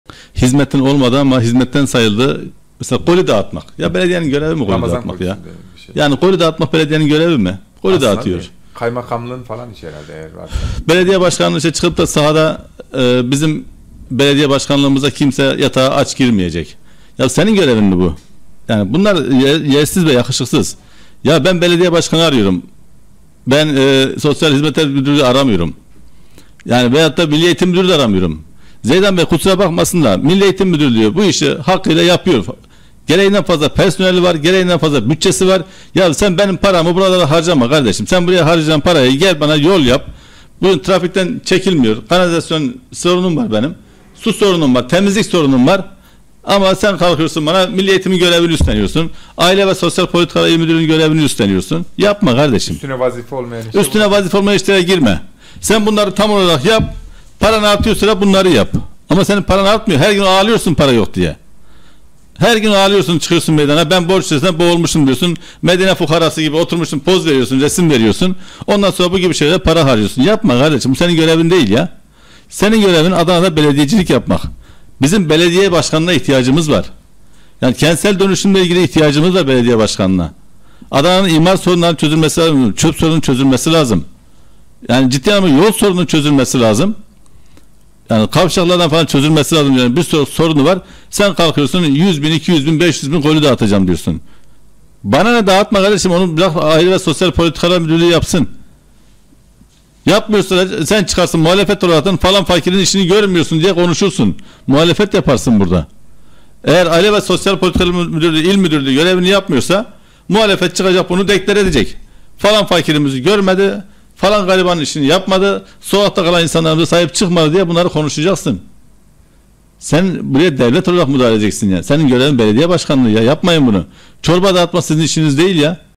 Zeydan başkan Medine fukarası gibi fotoğraf veriyorsun sonra üstüne vazife olmayan işler yapıyorsun!
Hüseyin Bayrak: Zeydan başkan Medine fukarası gibi oturup fotoğraf veriyorsun sonra üstüne vazife olmayan işler yapıyorsun!